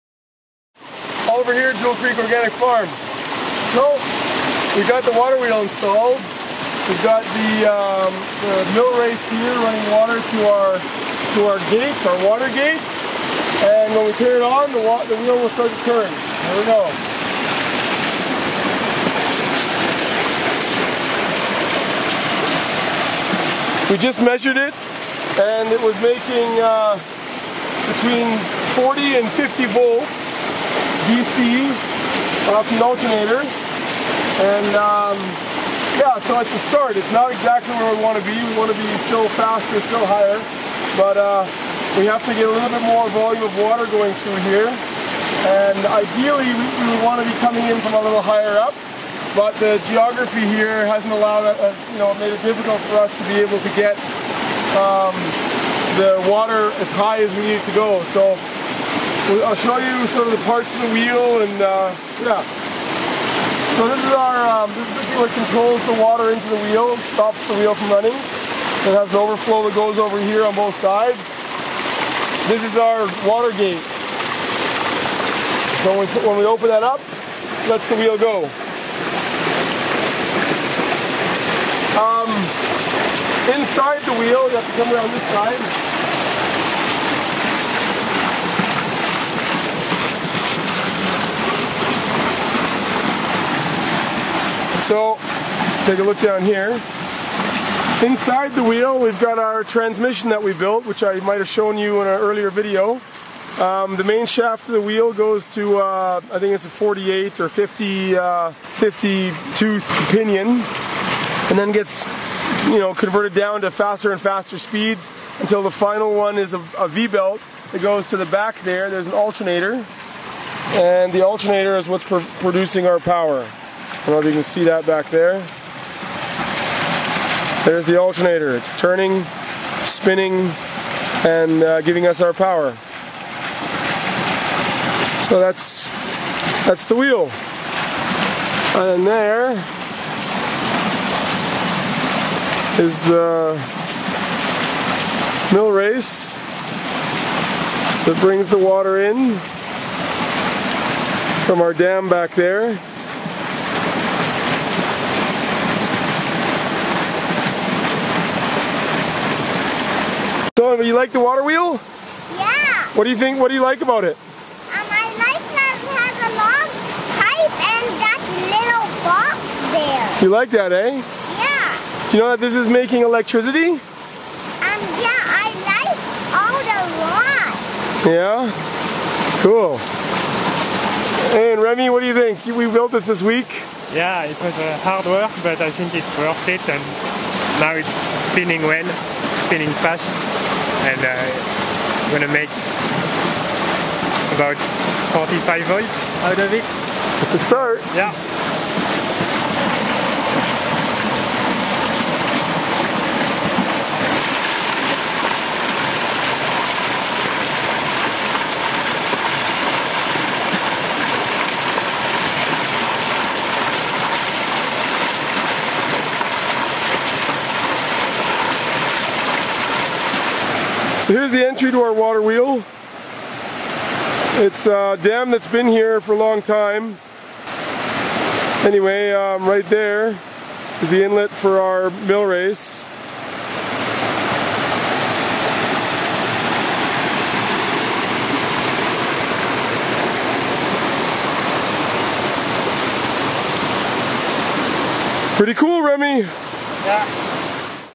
Waterwheel to produce electricity
A rebuild of our first waterwheel, this time with a transmission and alternator turning making power. built from a mix of wood, scavenged materials and a few chains, sprocket wheels and a PMA alternator we are producing 50V DC in this video. Shows pretty in depth all the parts of the wheel.